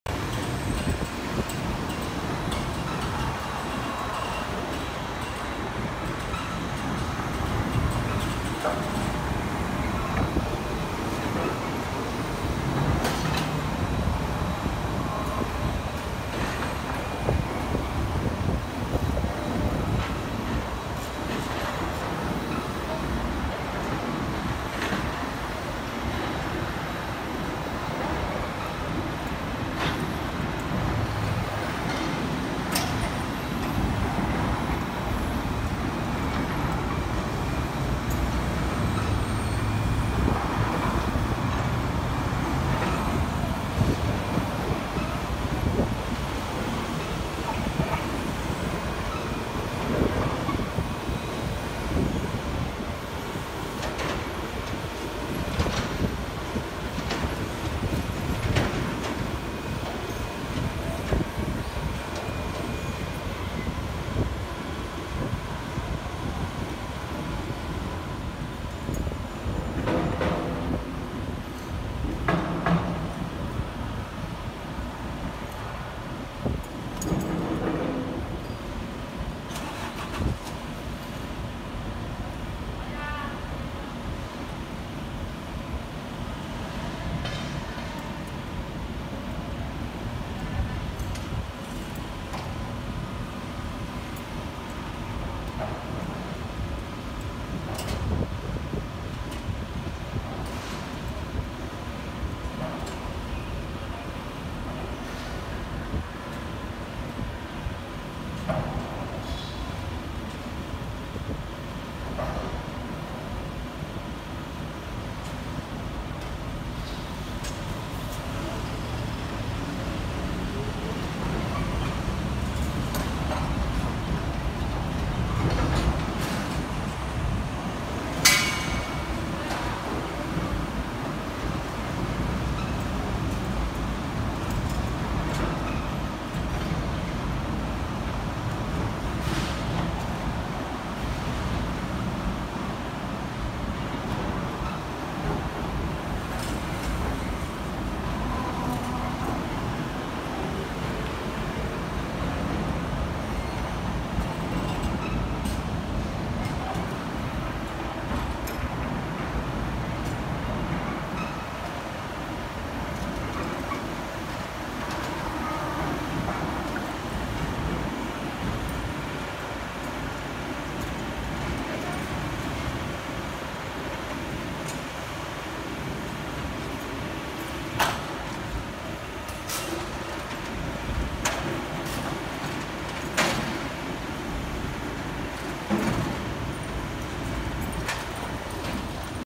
【渋谷の日常】まるでパズル！渋谷駅前のクレーン道路工事と採掘工事が同時に行っていた！深夜の工事現場！
東京オリンピックに間に合わせようと必死に工事してるんでしょうか？
終電前の渋谷駅の首都高高架下の道路工事です！
ショベルカーが大活躍です！